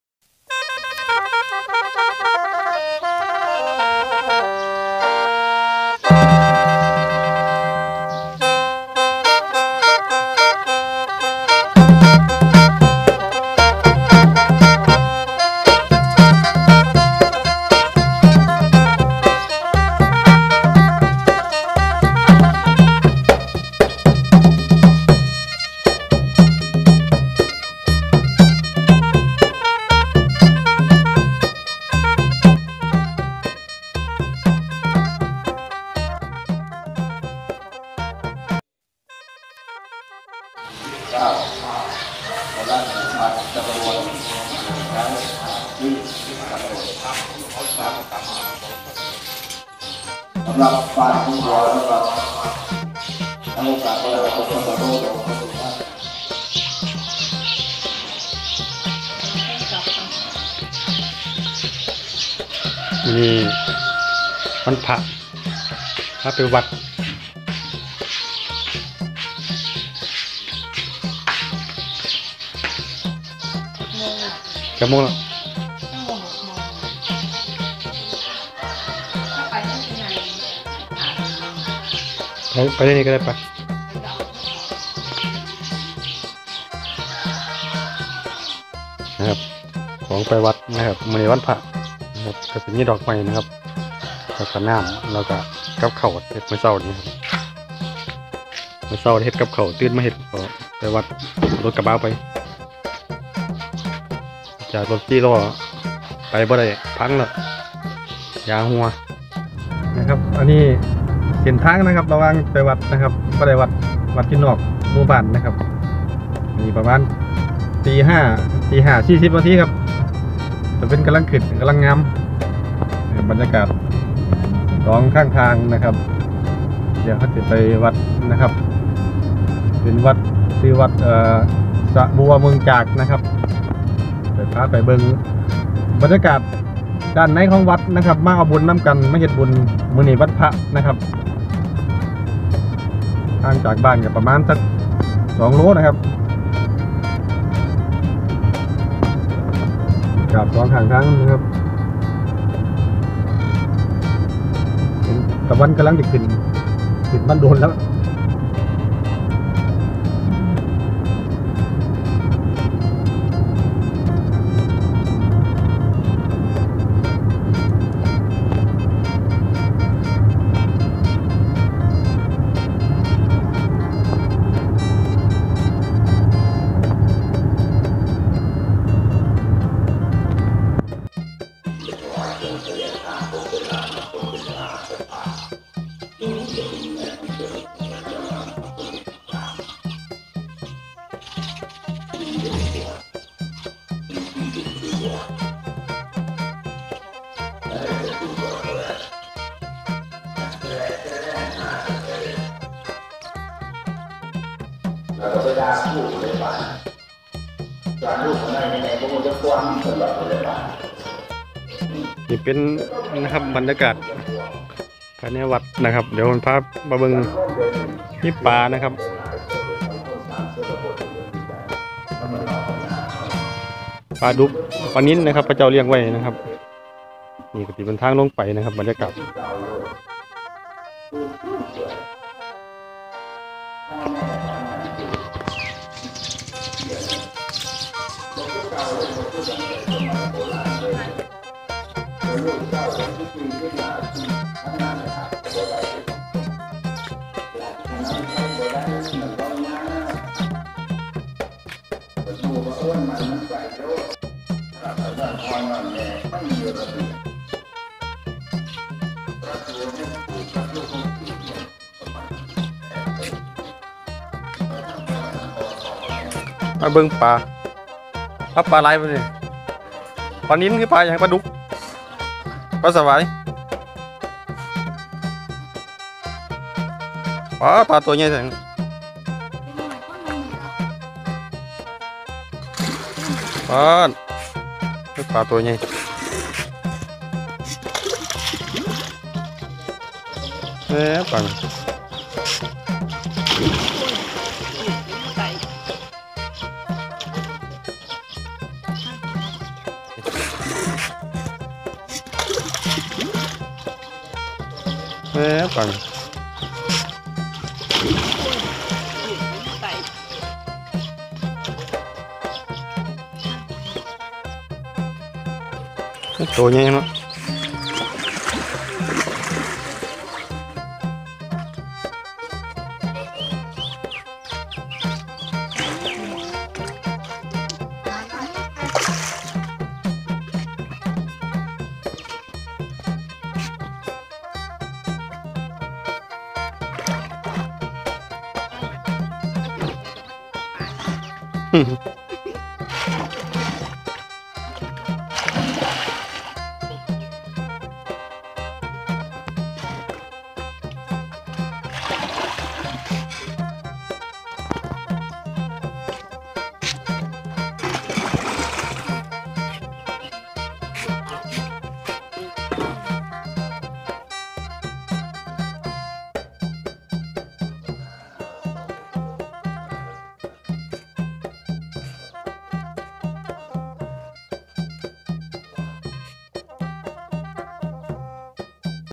ดูปลาใหญ่วัดสระบัวเมืองจาก ต.โนนอุดม อ.เมืองยาง จ.นครราชสีมา
วัดสระบัวเมืองจาก อ.เมืองยาง จ.นครราชสีมา
ตั้งอยู่ที่บ้านเมืองจากเป็นอีกหนึ่งวัดที่สวยงามในเขตอำเภอเมืองยาง#